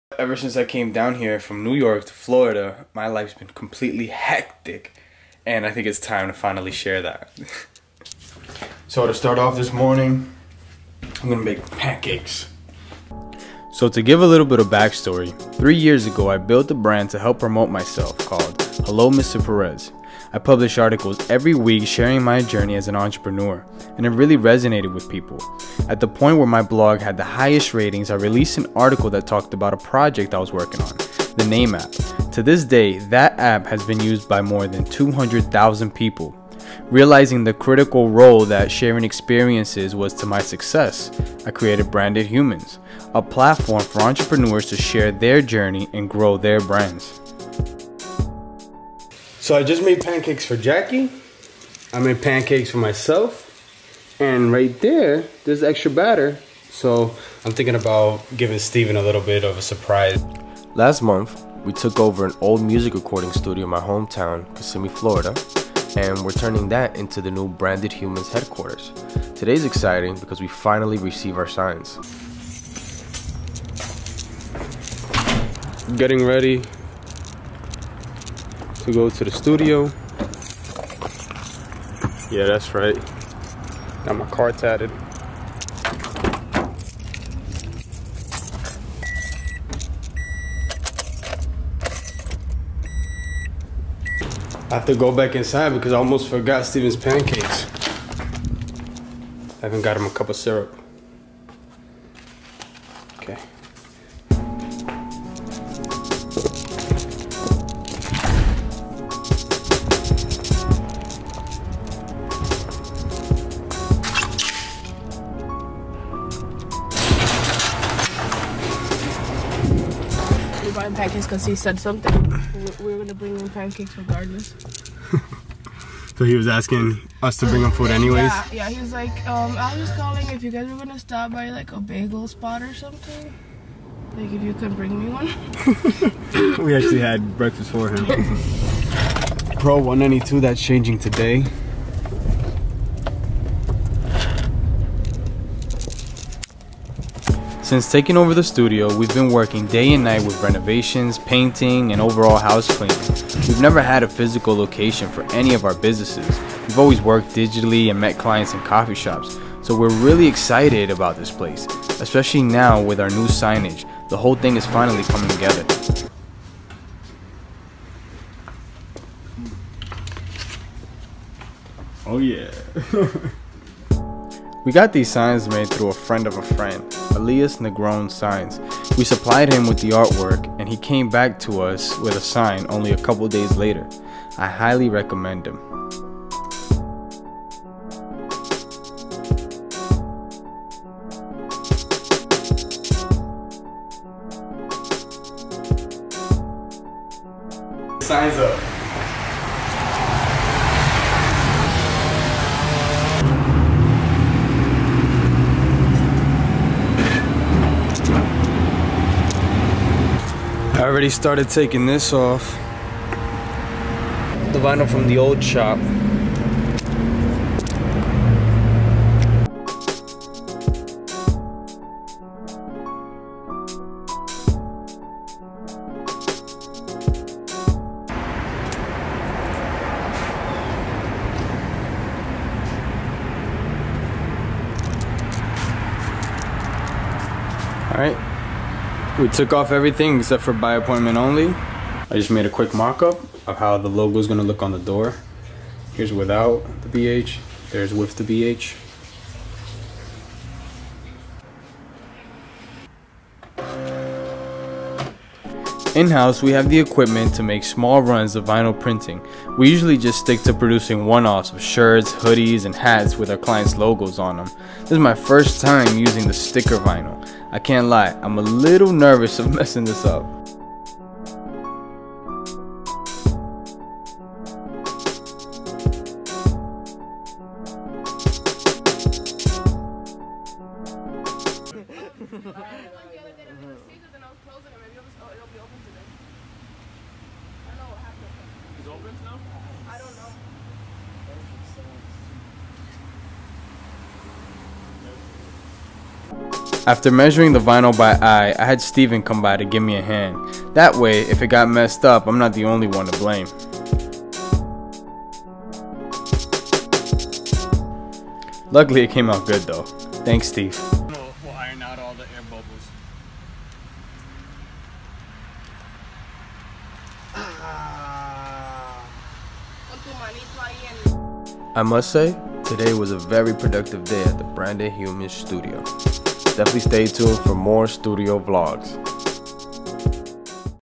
Signage Arrives for my New Start Up
After working two years on my startup in NYC, I received an opportunity to take over an old music recording studio in Florida. We've spent the last month turning it into the new Branded Humans Studio, and today the new signage arrives!